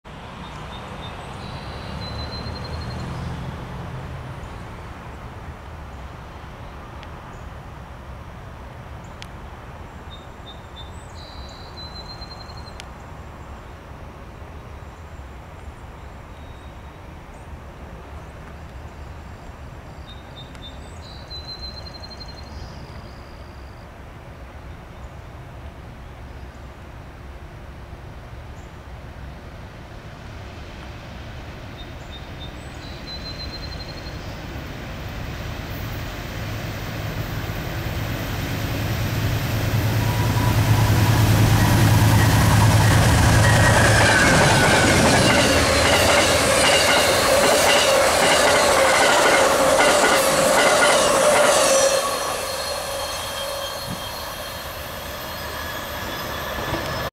Amtrak 19 Flying thru Doraville, GA 8/11/2024
Power: AMTK 14, AMTK 129
Speed: 75 mph
Location: CP Ray, Doraville, GA